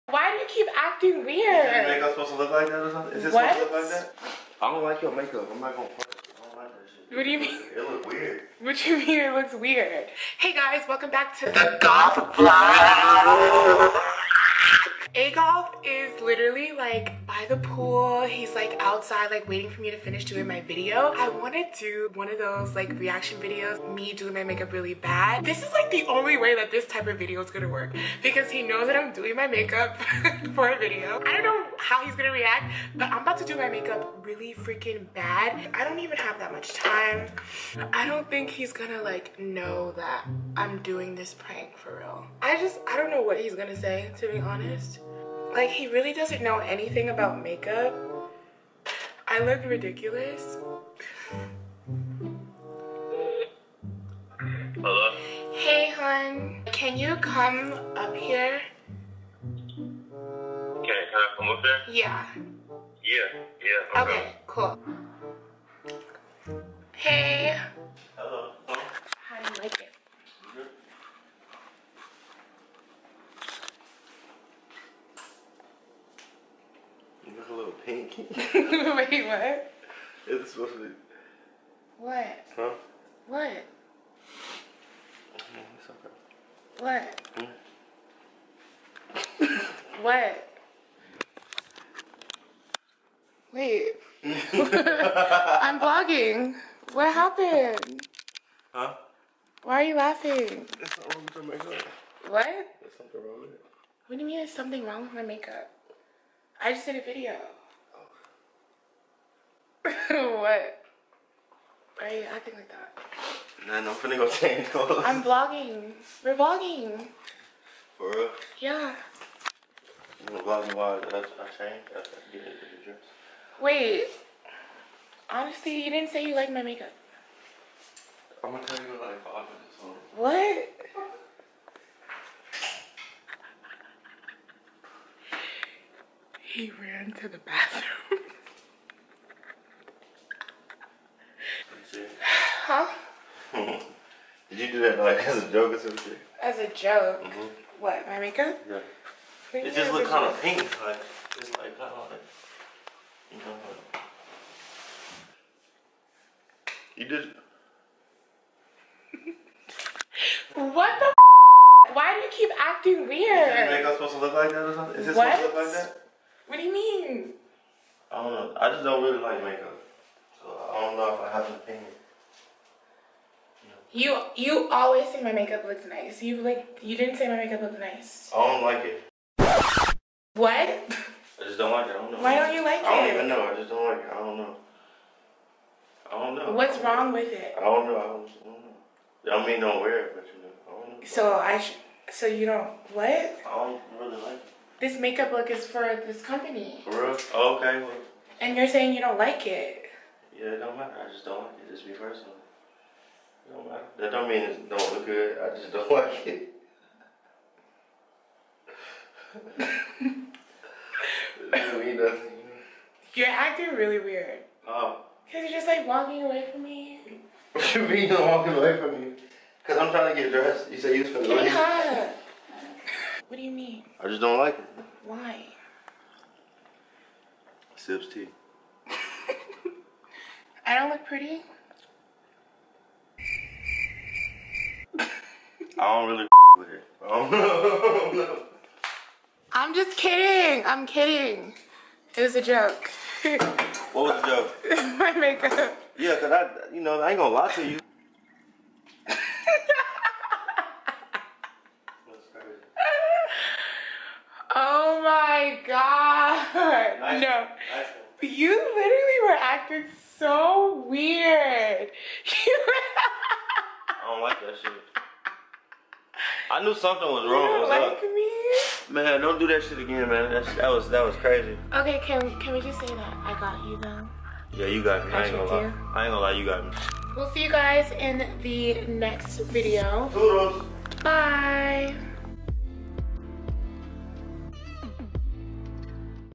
I DID MY MAKEUP HORRIBLY TO SEE HOW MY "BESTFRIEND" WOULD REACT PRANK! (HILARIOUS)
Hey ya'll so I decided to prank AGoff and do my makeup horribly his reaction was priceless!! Drop a comment if you enjoyed the video. See you next Tuesday!!